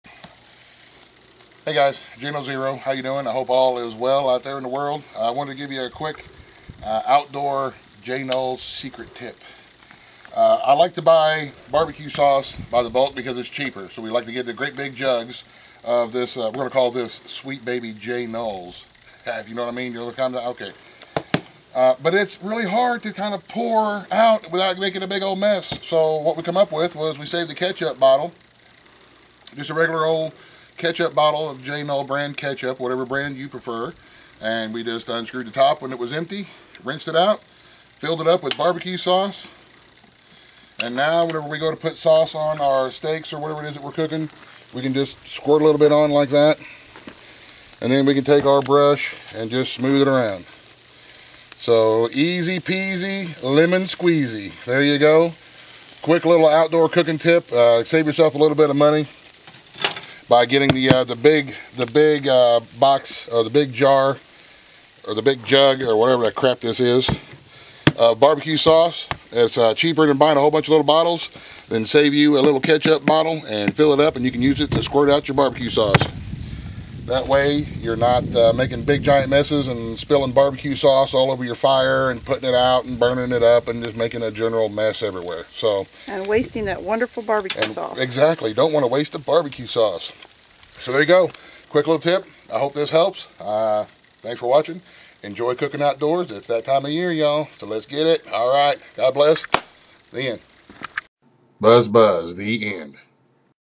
Quick BBQ Sauce Tip
Hey guys, just a quick tip for saving a little on BBQ sauce.  Hope this helps.
J